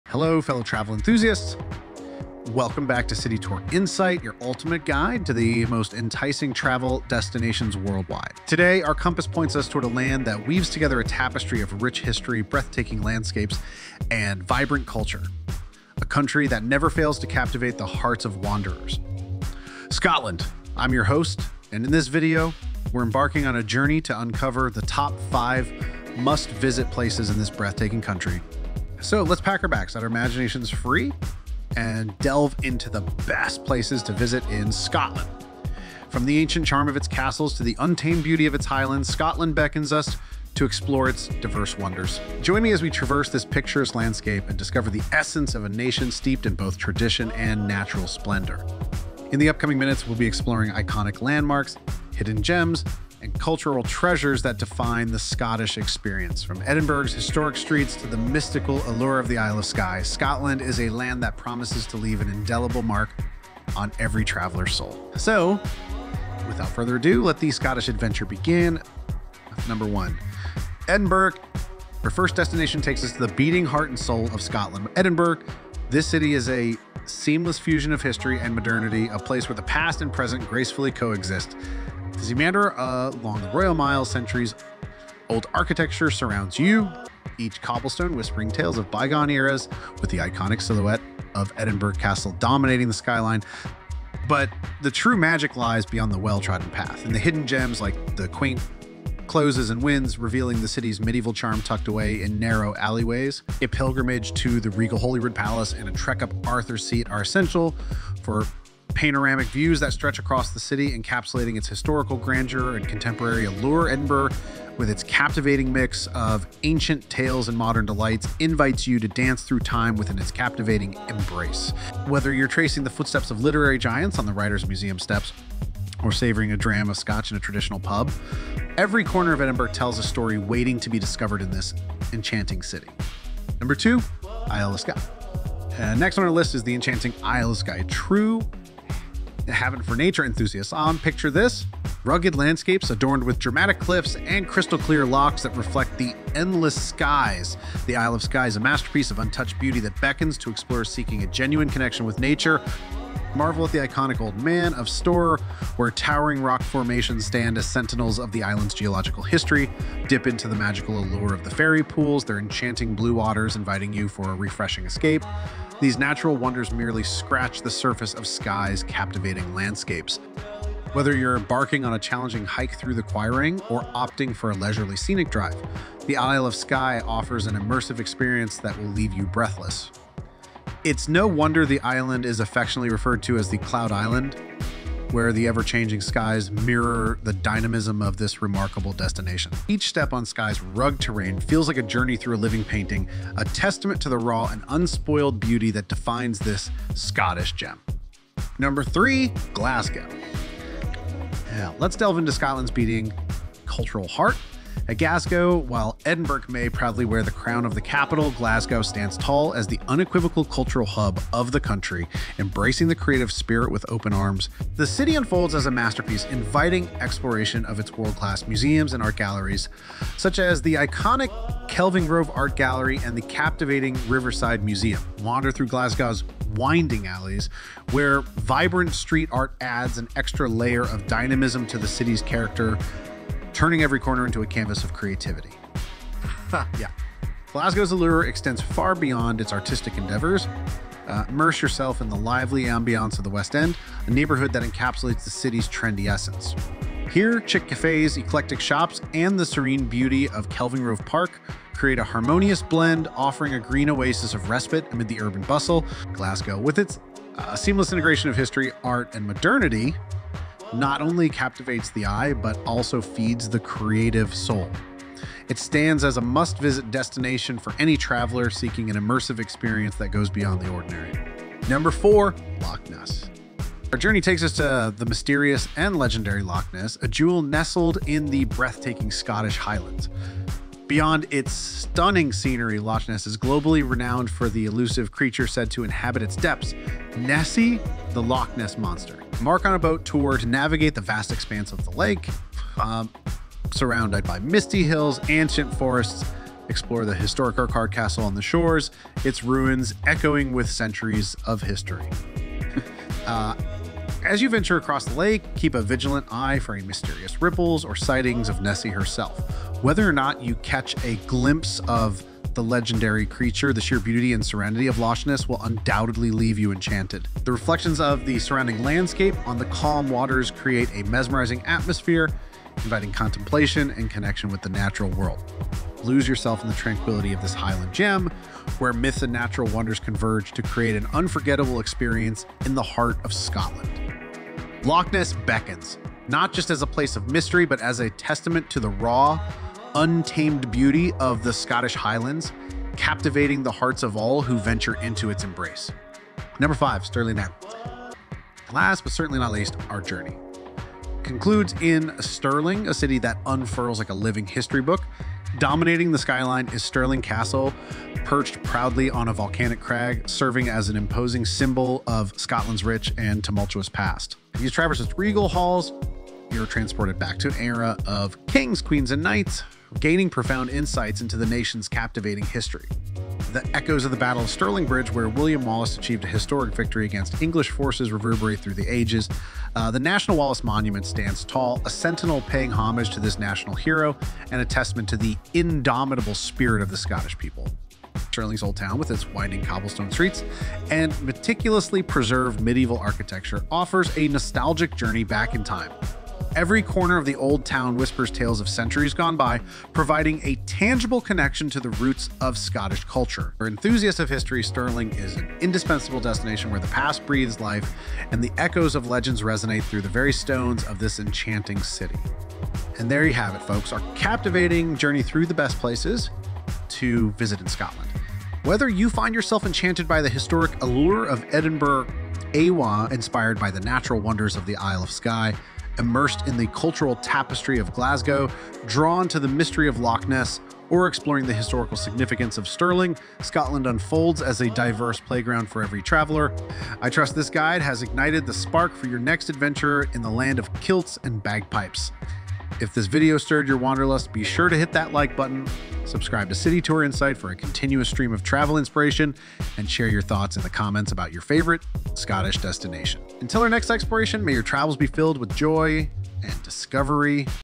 Unveiling Scotland's Top 5 Must-Visit Places in | City Tour Insight
Embark on a captivating journey with City Tour Insight as we unravel the enchanting beauty of Scotland's top destinations! 🏴󠁧󠁢󠁳󠁣󠁴󠁿✨ In this immersive travel guide, join our host as we traverse the historic streets of Edinburgh, where ancient tales blend seamlessly with modern delights. Explore the iconic Edinburgh Castle, hidden closes, and wynds that reveal the city's medieval charm. Move on to the Isle of Skye, a haven for nature enthusiasts, where rugged landscapes, dramatic cliffs, and crystal-clear lochs create a masterpiece of untouched beauty.

Glasgow, the cultural heart of Scotland, invites you to wander through world-class museums, vibrant street art-filled alleys, and the trendy West End. The mysterious Loch Ness beckons with its legendary monster and serene reflections, offering a tranquil escape in the heart of the Scottish Highlands. Finally, our journey concludes in Stirling, a living history book dominated by the regal Stirling Castle and the echoes of the Battle of Stirling Bridge.

Whether you're drawn to history, nature, or cultural experiences, Scotland unfolds as a diverse playground for every traveler. Let City Tour Insight be your guide to the wonders of Scotland in 2023! 🌍🔍